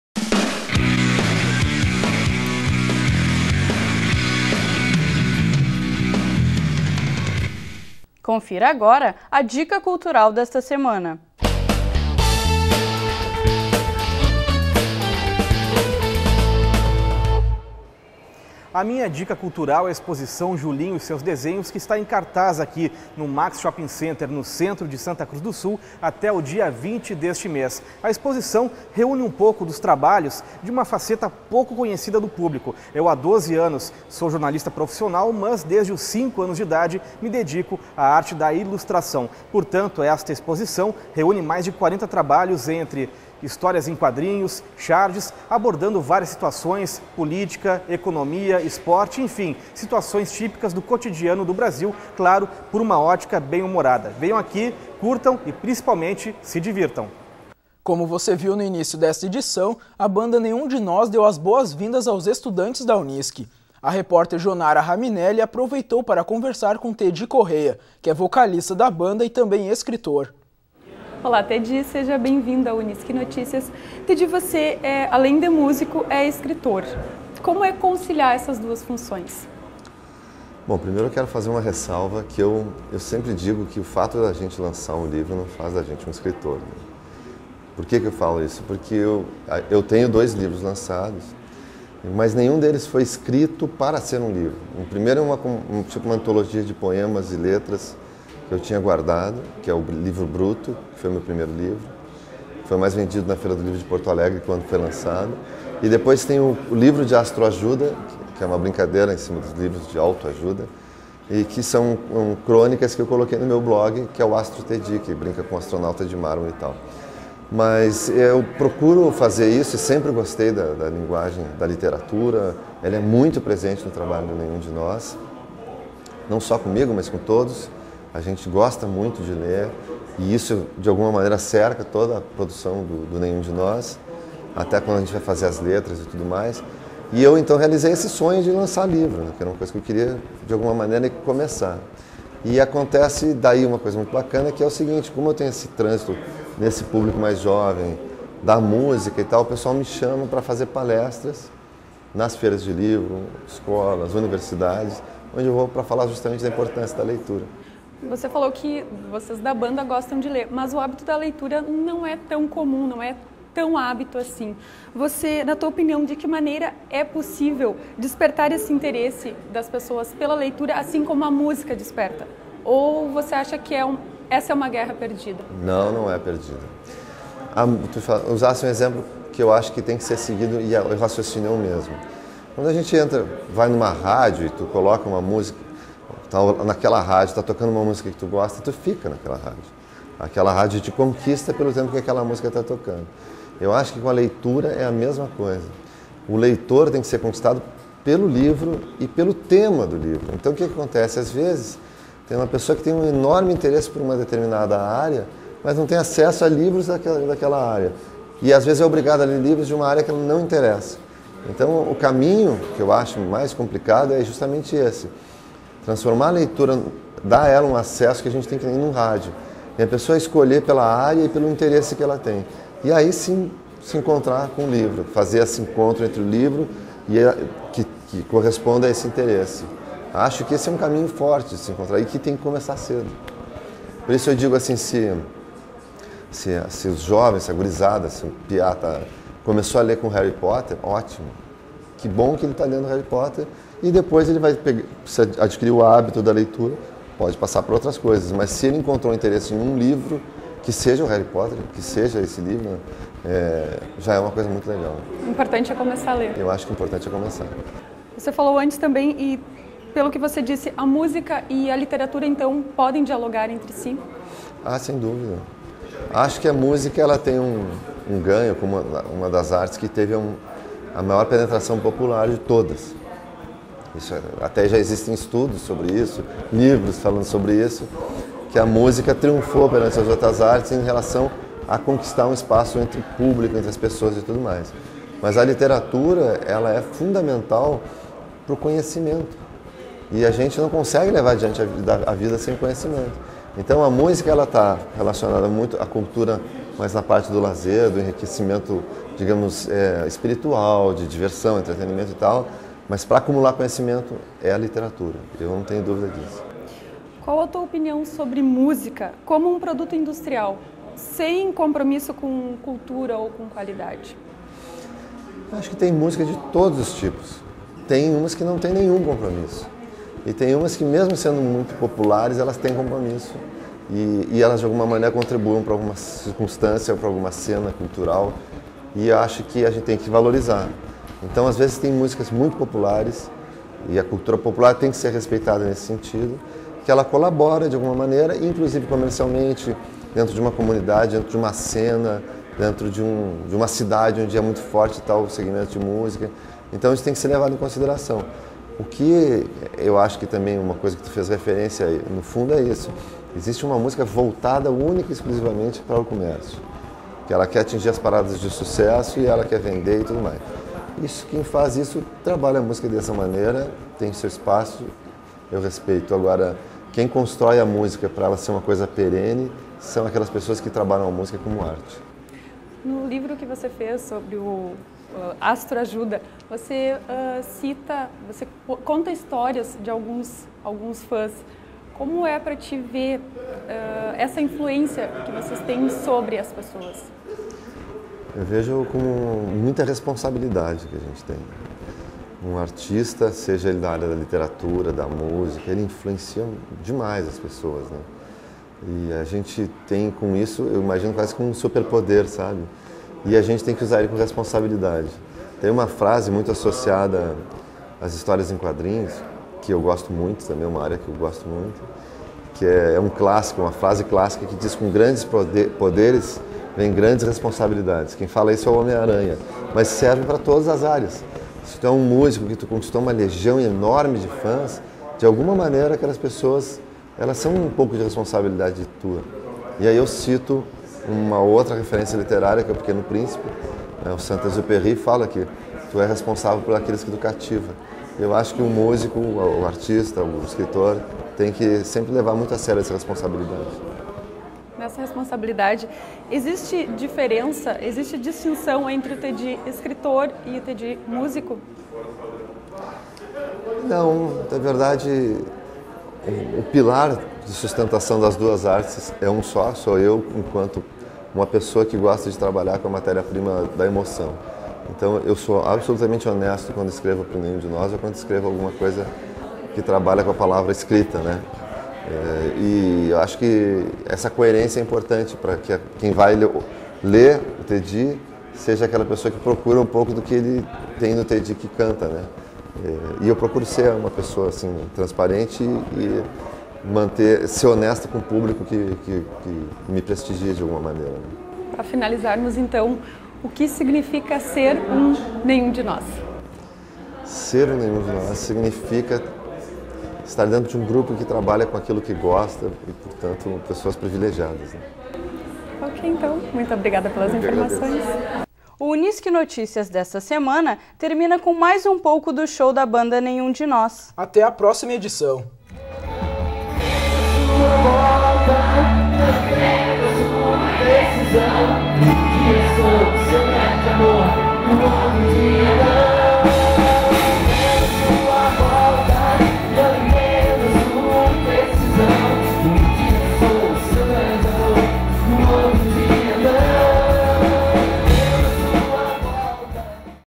Unisc Notícias - 16/03/2012 - Bloco 3
- Dica Cultural,
- Entrevista com o vocalista da banda Nenhum de Nós, Thedy Correa.